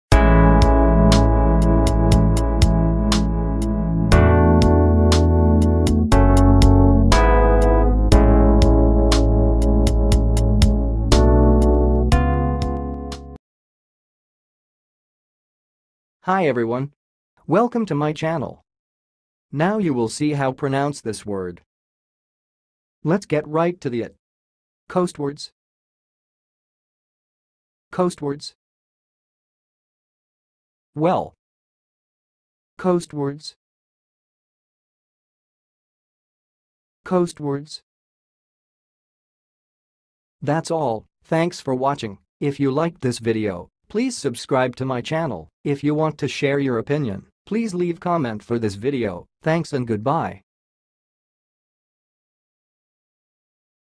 COASTWARDS - HOW TO PRONOUNCE IT!?
Watch how to say and pronounce "coastwards"!
Want to know how other words sound like? Look for coast, coastwards on wordbox channel!